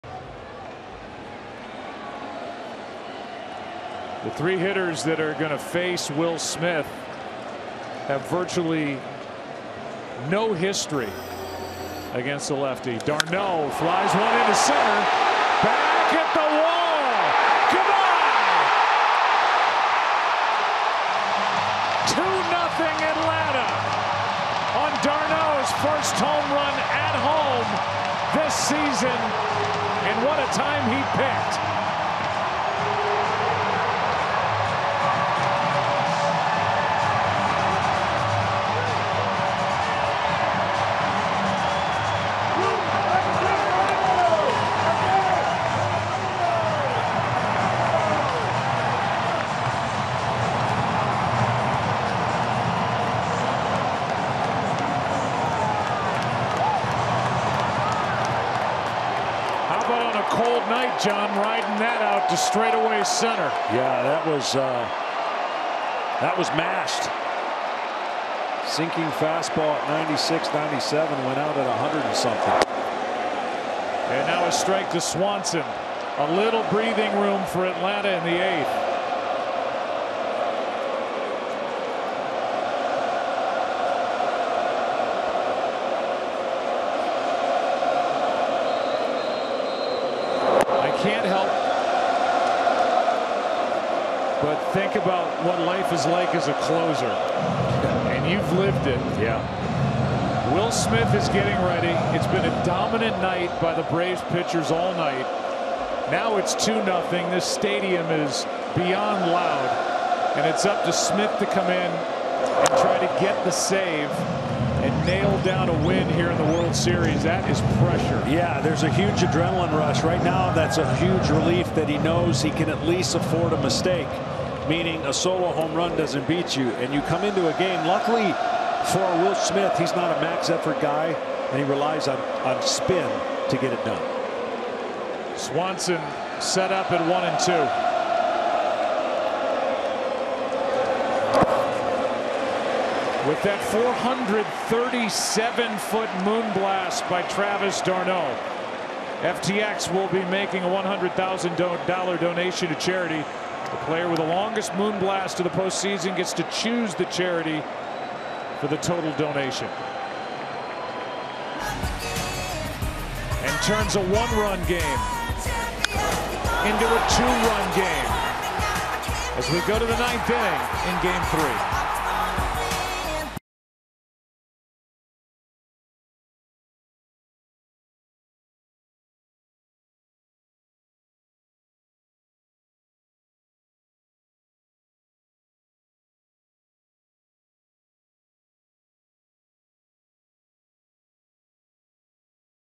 Travis d'Arnaud goes DEEP to give the Braves some insurance in the 8th!
d'Arnaud hits one 437 ft to give the Braves a 2-0 lead.